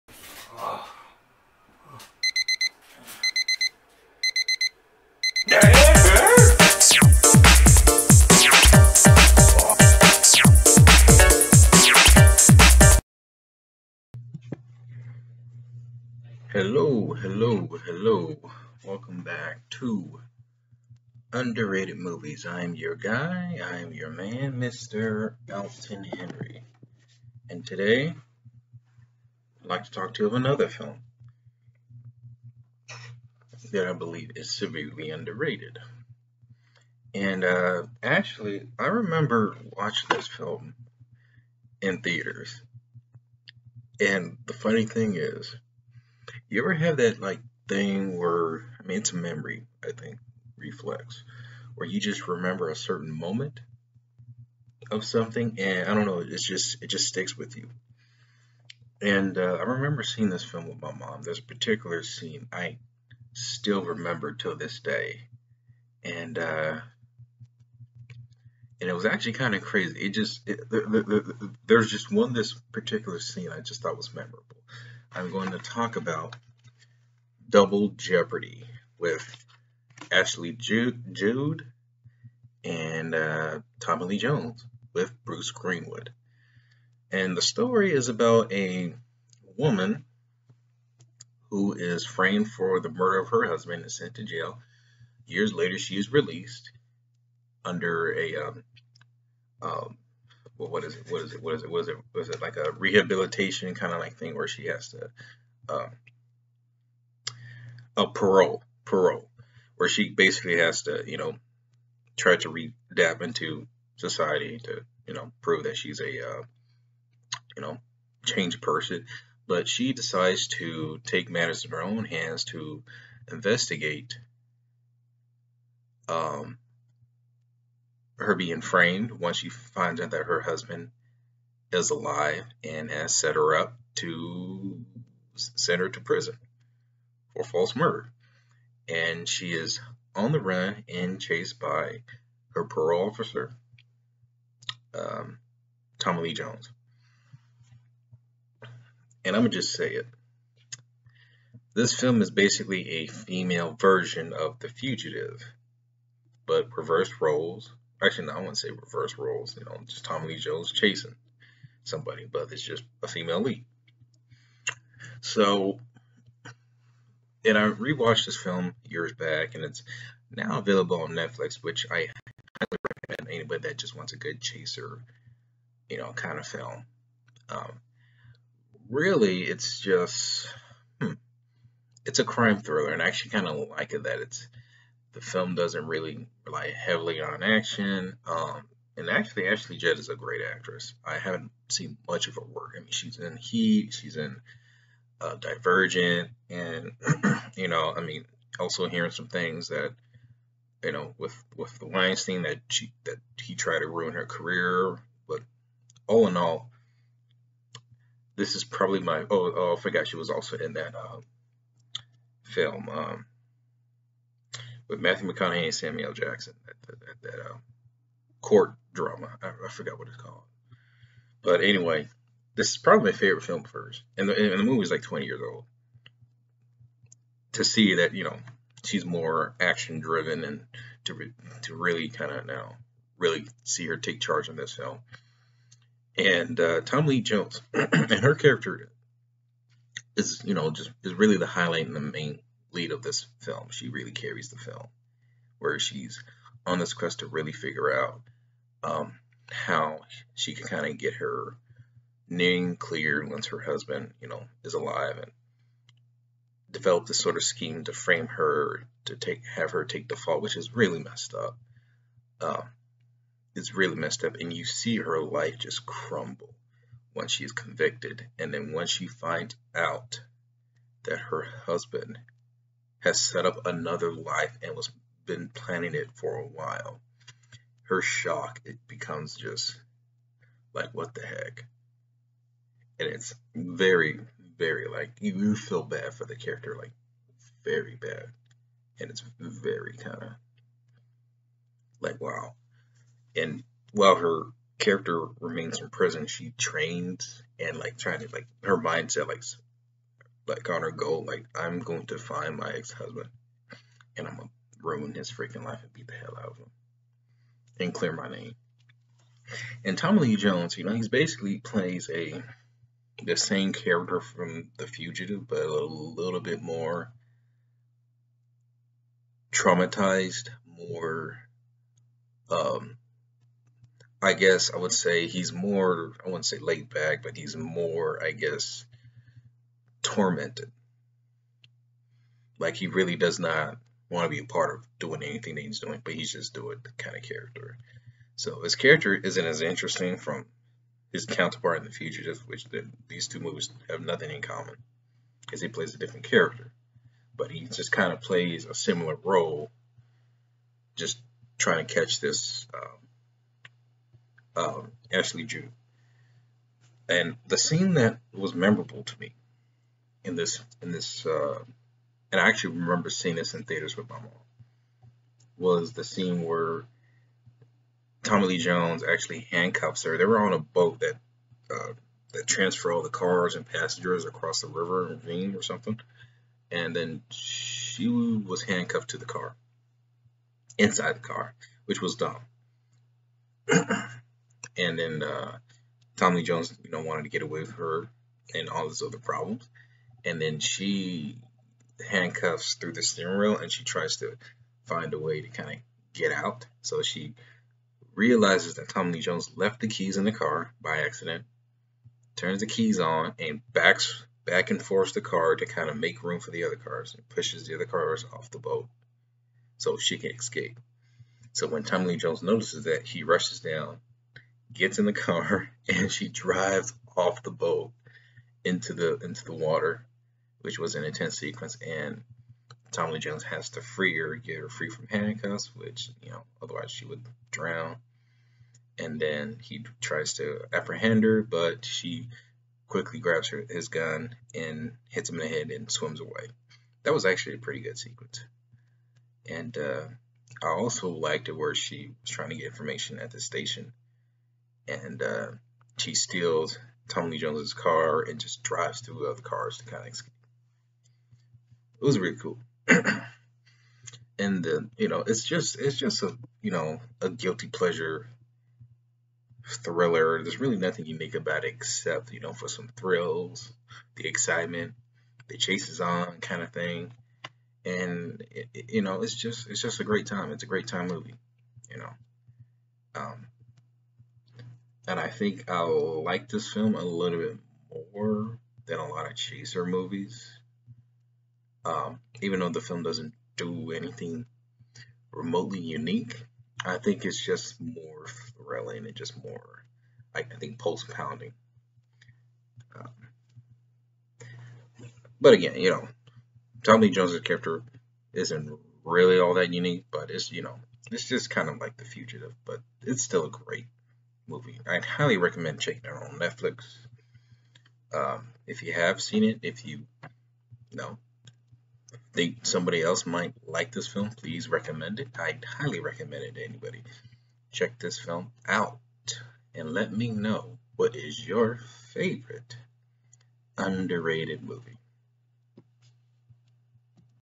Underrated Movies- Double Jeopardy
Released in 1999,  the film follows a woman framed for her husband's murder, and suspects he's still alive after already being convicted of the crime.  She sets out on a quest of vengenance and to clear her name while also being pursued by her parole officer.